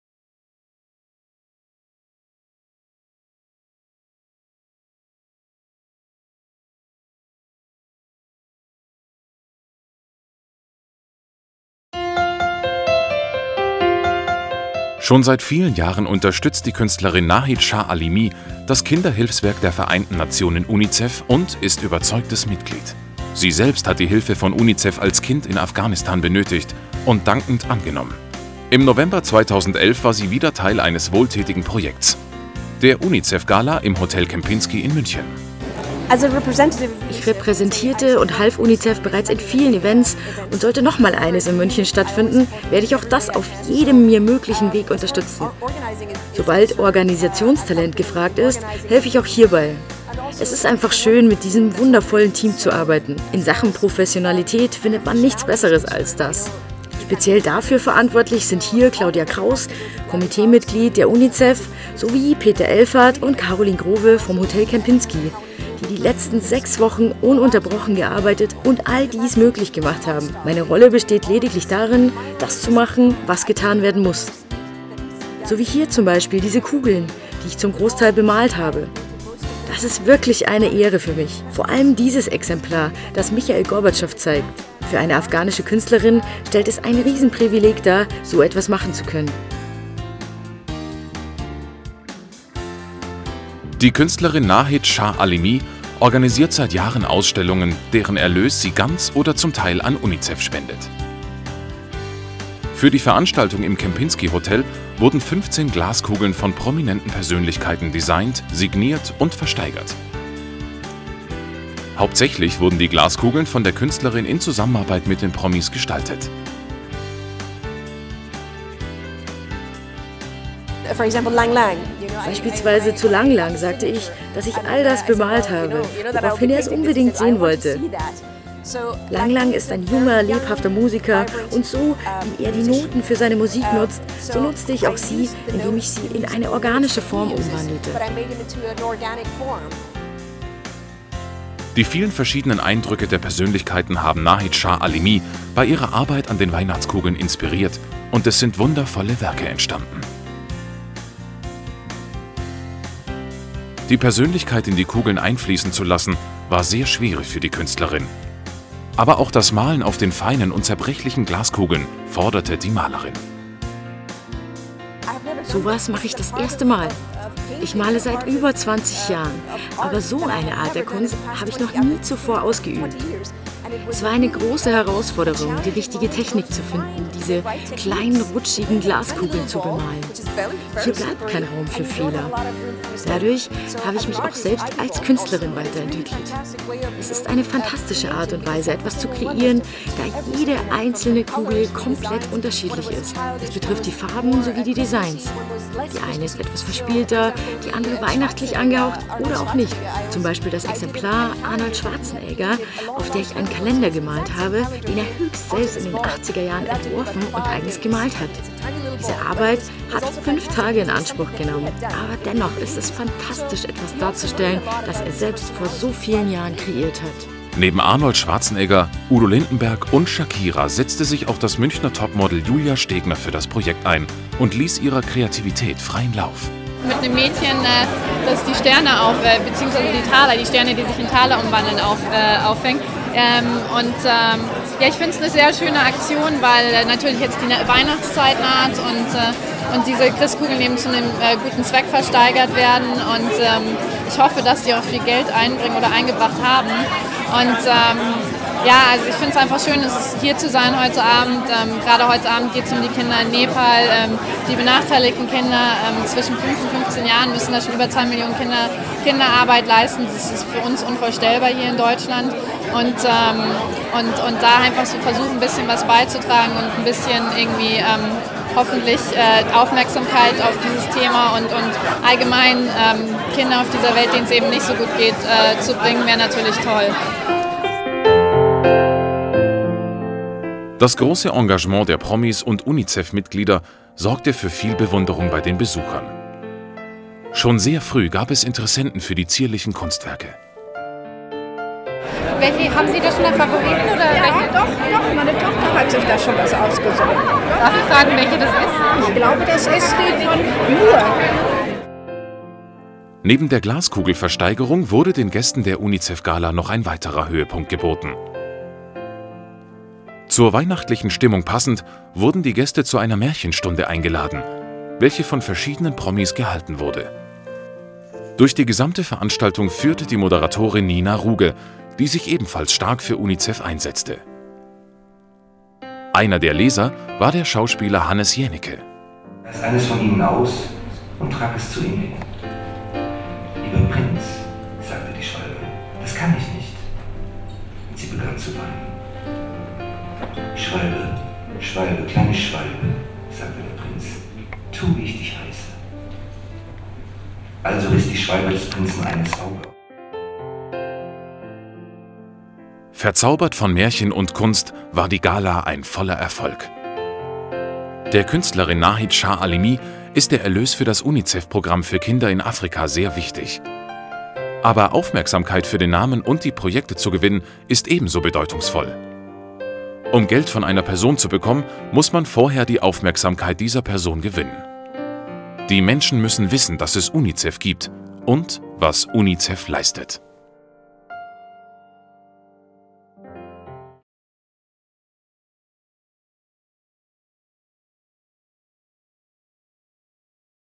Nahid Shahalimi - UNICEF Gala Munich
Christmas Bulbs' Auction, hand painted by Nahid Shahalimi and signed by; Lang Lang, Shakira, Gorbechev, Roger Moore, Christopher Lee, Ana Netrebko, Julia Stegner, Arnold Schwarzenegger...